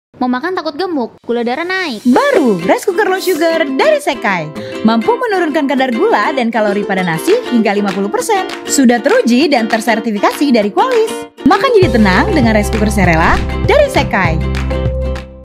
Digital Rice Cooker Low Sugar SEKAI Serela - Ads 15 Second
Baru Digital Rice Cooker Low Sugar SEKAI Serela CMW 720 LS. Mampu menurunkan kadar gula dan kalori hingga 50%, sudah tersertifikasi Qualis. Garansi service seumur hidup.

Pilih Rice Cooker
Sekai Pilihanku

Bisa didapatkan di official store sekai di marketplace kesayangan kalian.